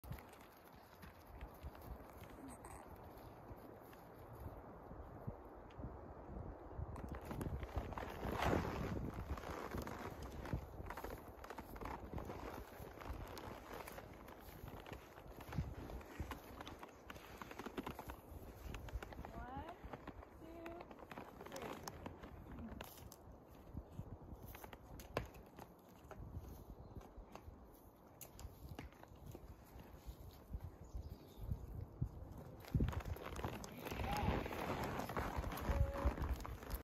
Parallel Parking - Level 3 with Ally
Parallel Parking obstacle
Level 3: From start cone, walk to cone, stop. Yield hind end over to position equine to start backing into parking spot. Back equine into parking spot and center. Settle 3 seconds. Sidepass out of parking spot and exit.

Riverside State Park
May 7, 2022
Spokane, WA